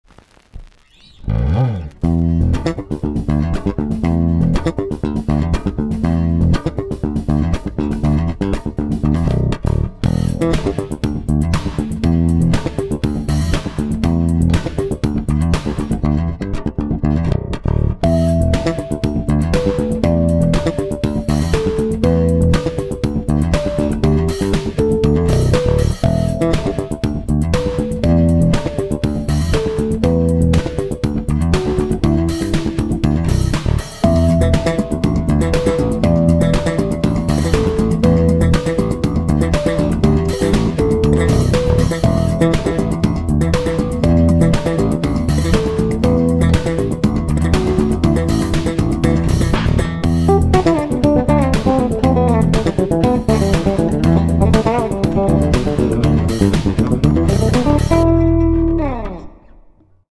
Camarota Bass "Polaris II Deluxe 5" (Emanuele Simoncini)
Camarota Instruments Polaris II, cocobolo top, swamp ash body, indian rosewood 24 frets fingerboard, cocobolo pots, MAMA Omnia dual coil pickups mama pickups, custom Markbass 3 - band preamp with passive tone, Hipshot hardware, Markbass strings Longevo series